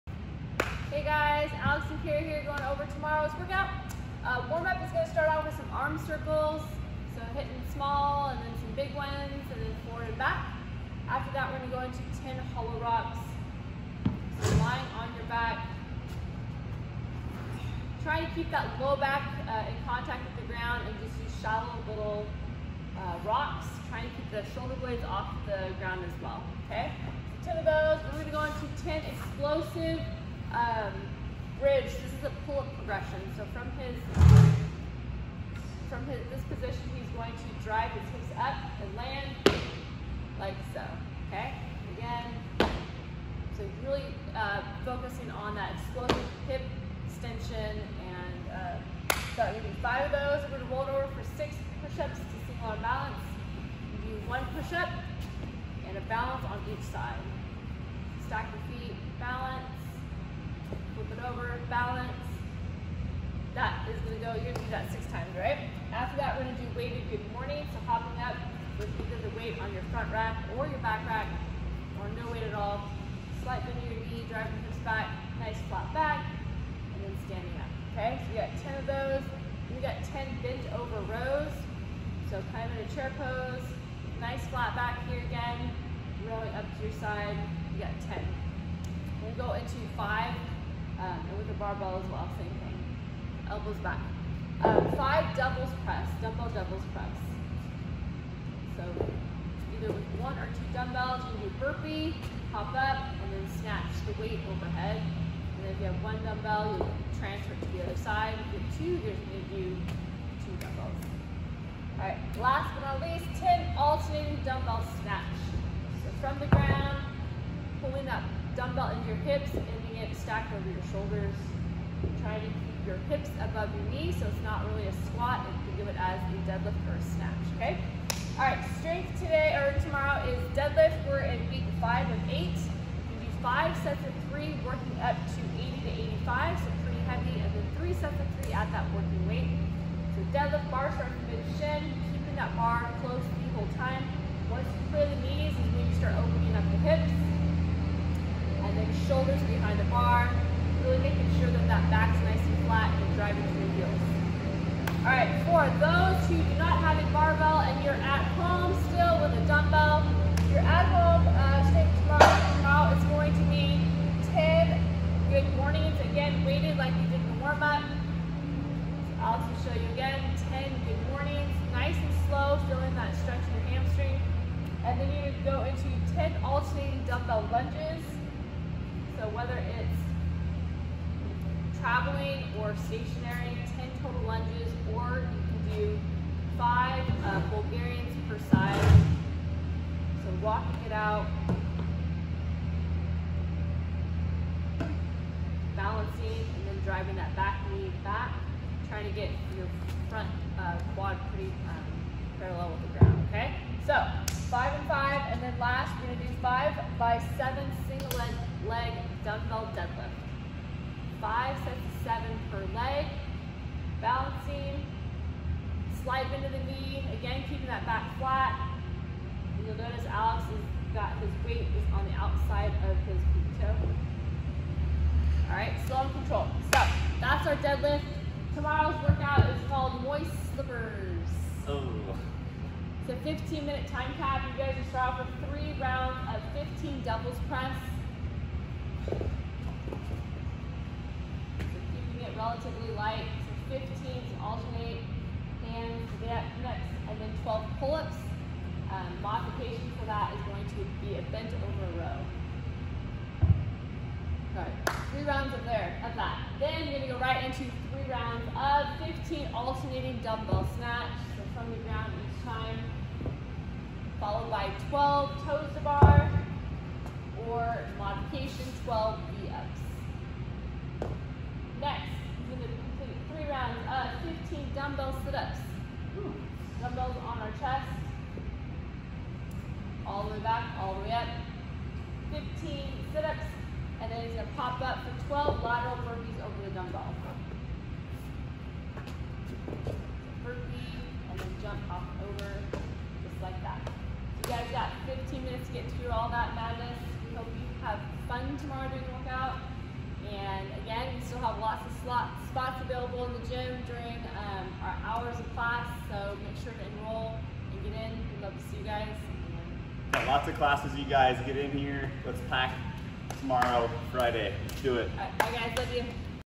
7.10.20 Workout Description - CrossFit Chateau
7.10.20 Workout Description - CrossFit Chateau
Strength: Deadlift
@ Home Strength: Goodmornings, Lunges/Bulgarians, SL DL
WOD: Devils Press, Pull Ups, DB Snatch, TTB, DB Sit Ups, Lateral Burpee Over DB
Schedule & Sign In
*Download the Pike13 App to log into classes easier*